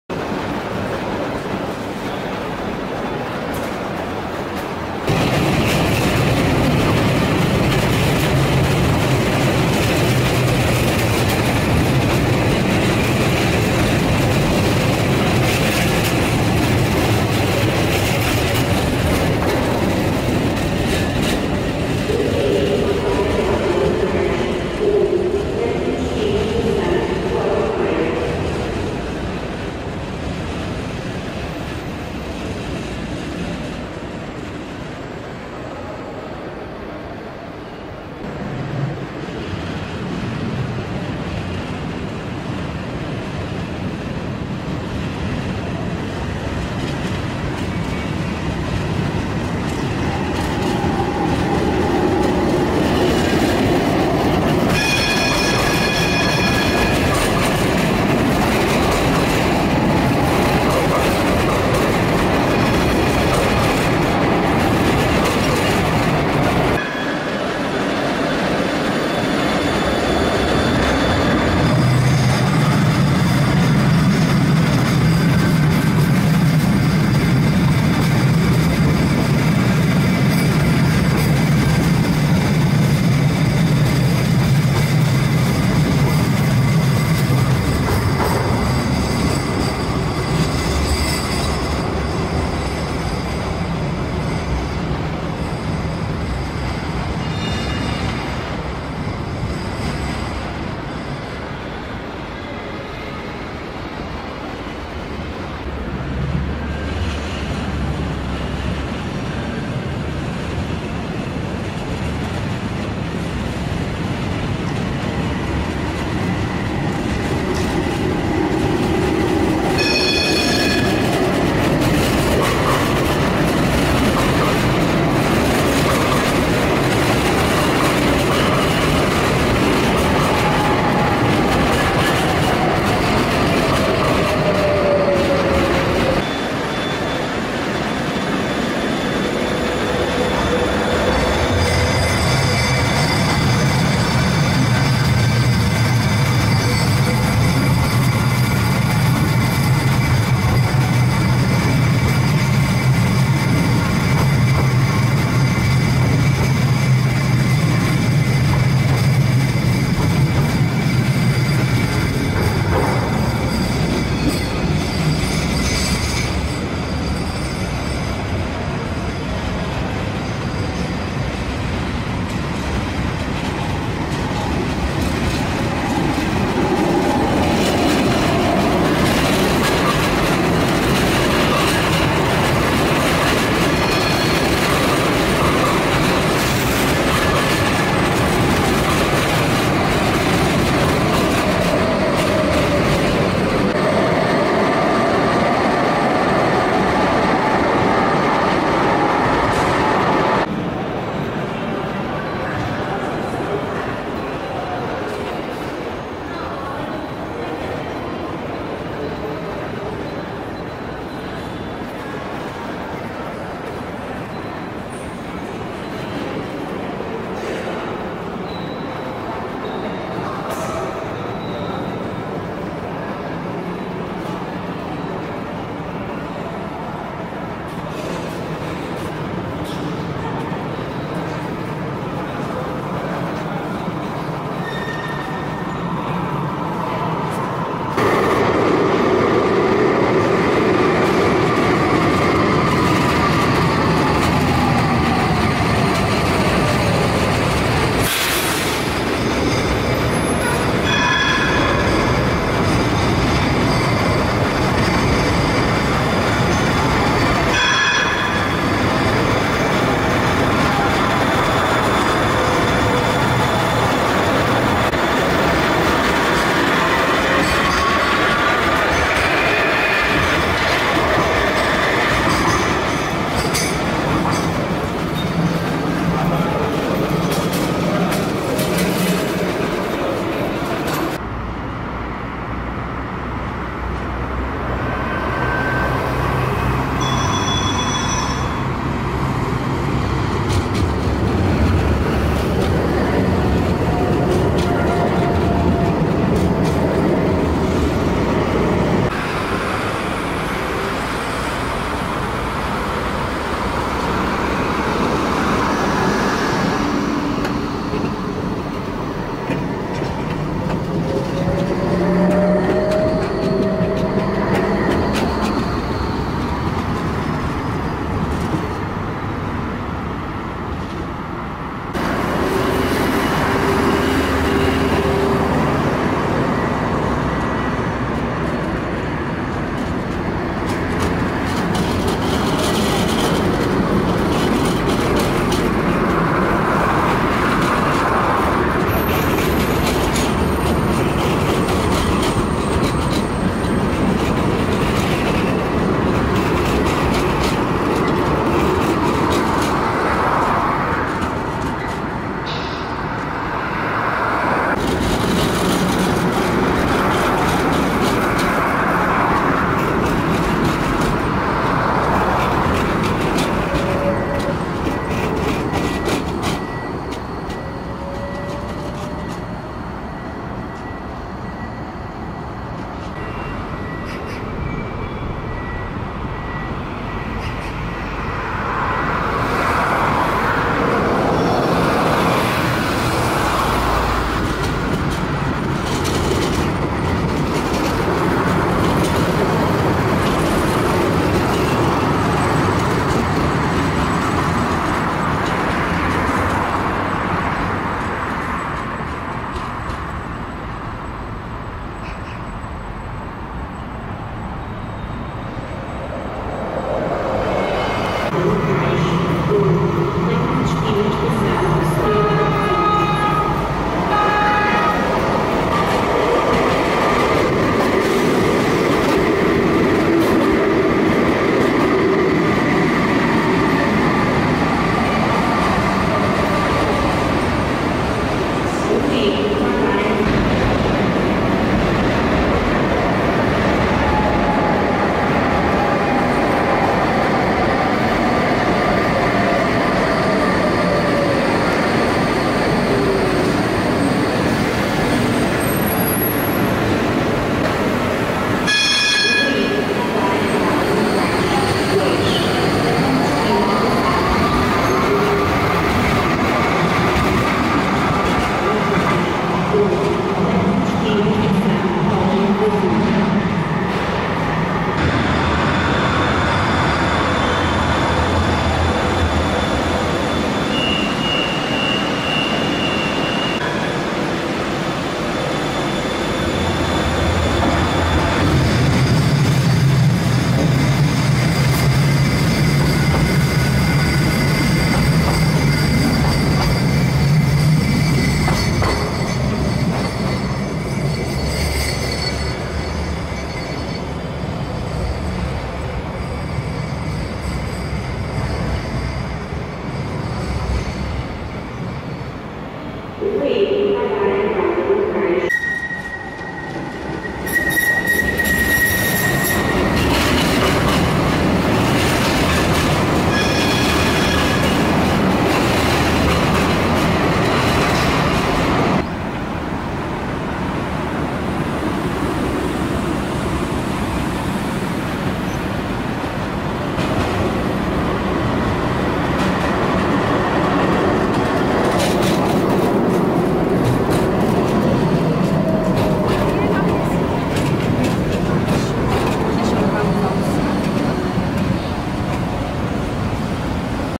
Porto Metro - November 2018
Clips of the Porto Metro light rail system
Trinidade station
Senhor de Matosinhos station and terminus
Mercado station